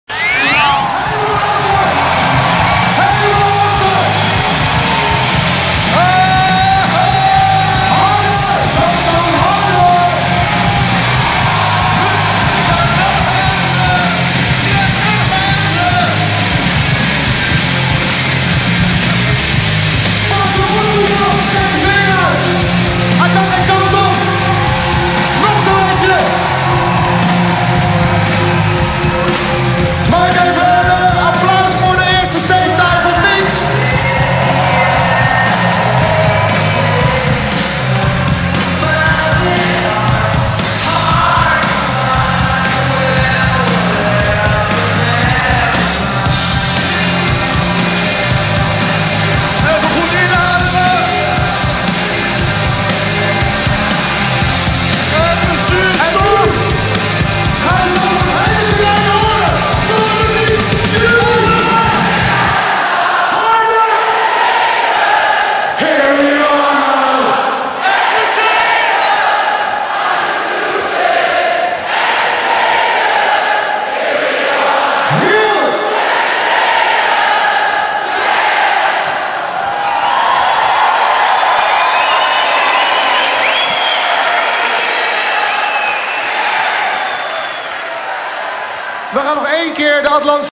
lowlands2009 "lowlands zingt"
alpha lowlands 22 augustus 2009 13:00 uur
Lowlands zingt
Martin Haring crowdsurfen